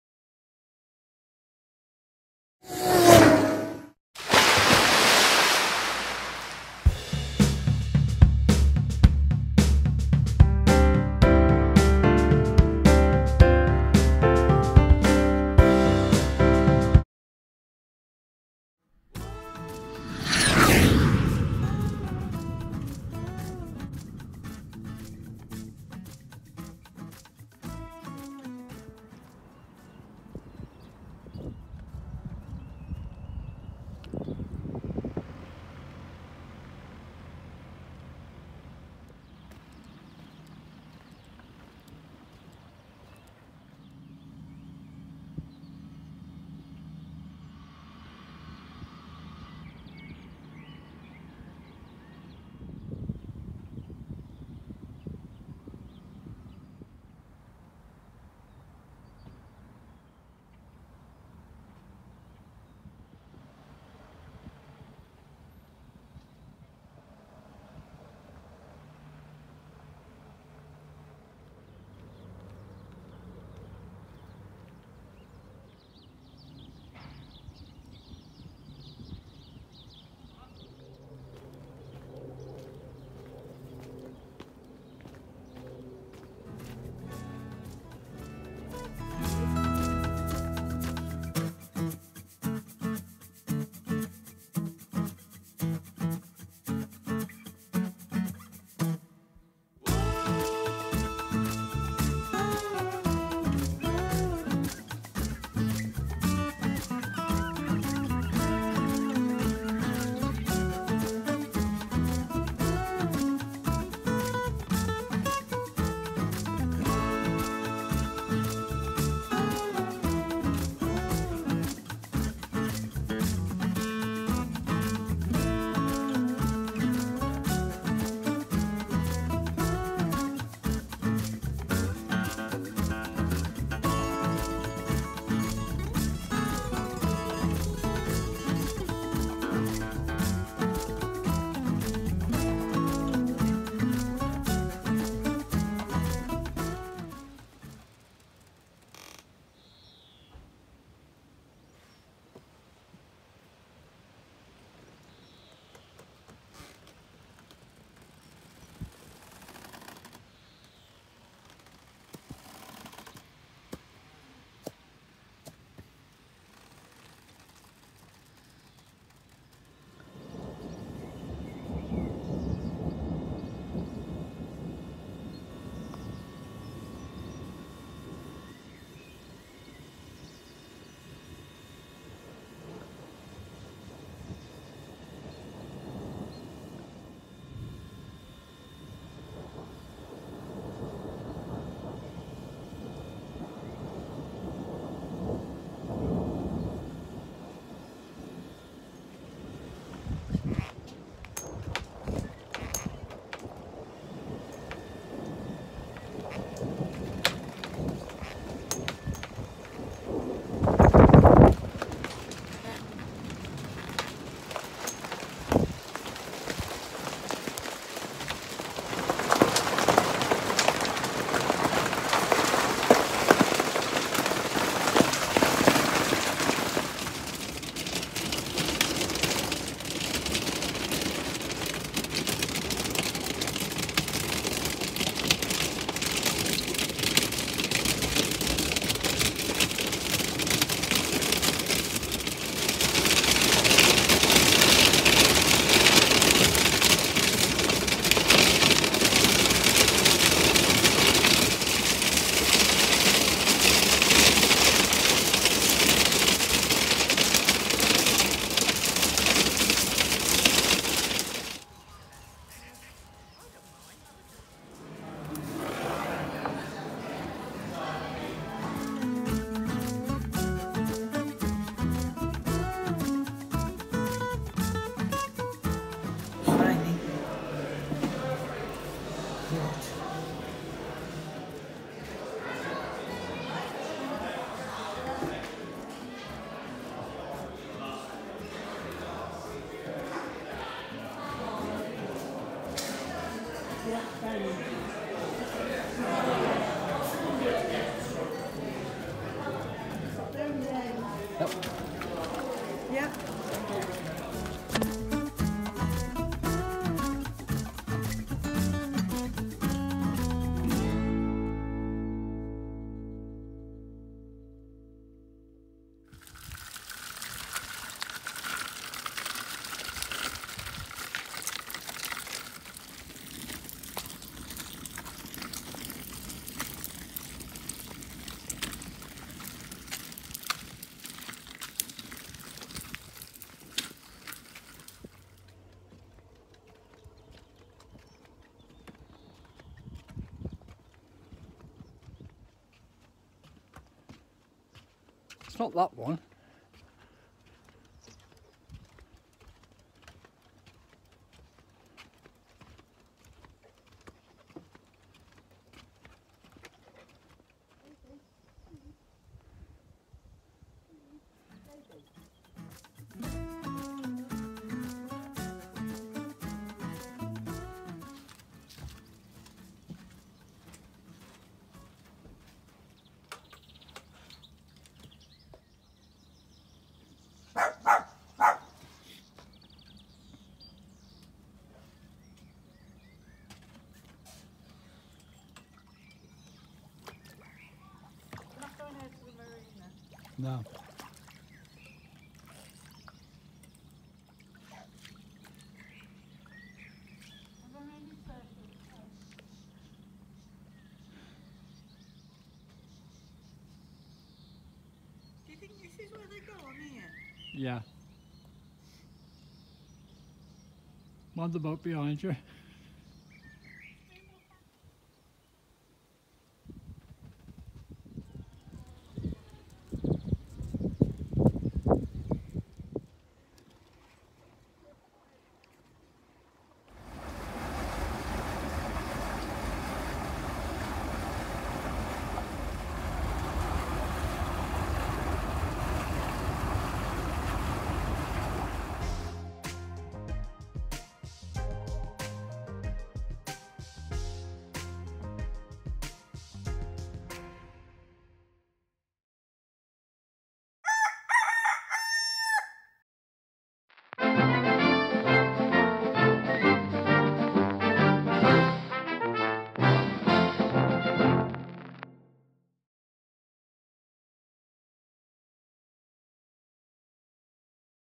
A Bit of This, That and T’other… May Bank Holiday - 2023
What have we been up to these past few weeks? To start off we had a couple of nights at Warwick Racecourse in Verity before heading off to Lady Jane at our Brundall Mooring. We had 2 weeks on board and we were determined that the last few jobs needed to get her BSS Certificate would be completed. Would we succeed? There’s also a visit to the local pub, Muffins new stroller and a maiden voyage 😀